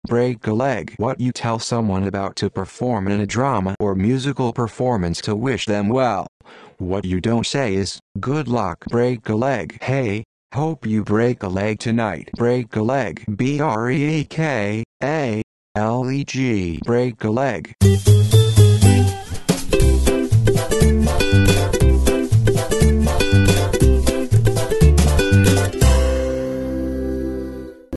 break a leg meaning and pronunciation
What you tell someone about to perform in a drama or musical performance to wish them well.
What you don't say is "good luck".

break a leg definition by Urban Dictionary